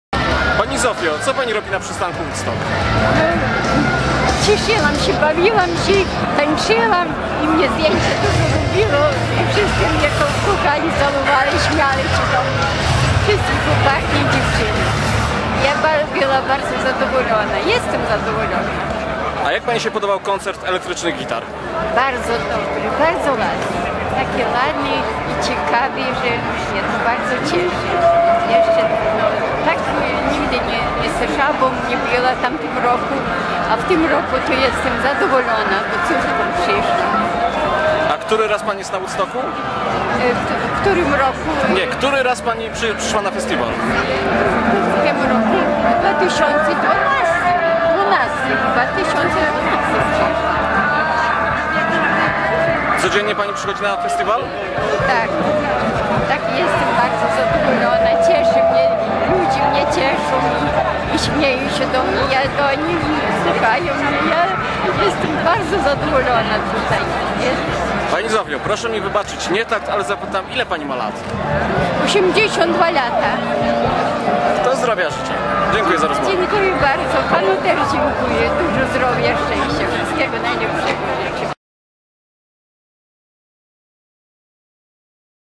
82-letnia woodstockowiczka
Trzeciego dnia Przystanku Woodstock w pobliżu sceny spotkaliśmy Zofię Olejnik. 82-letnia kostrzynianka, jak mówi, bawiła się na fesiwalu wyśmienicie.